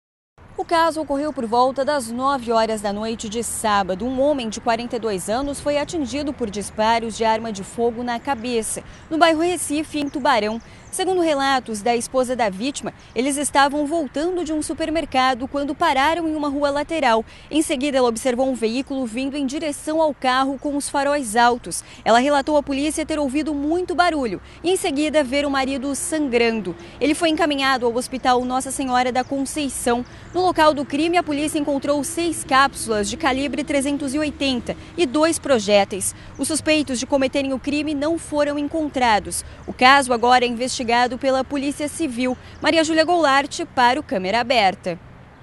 Homem de 42 anos é atingido por disparos de arma de fogo
Um homem de 42 anos foi baleado na noite de sábado, no bairro Recife em Tubarão. Ele estava com o carro parado em uma rua do local, quando foi atingido pelos disparos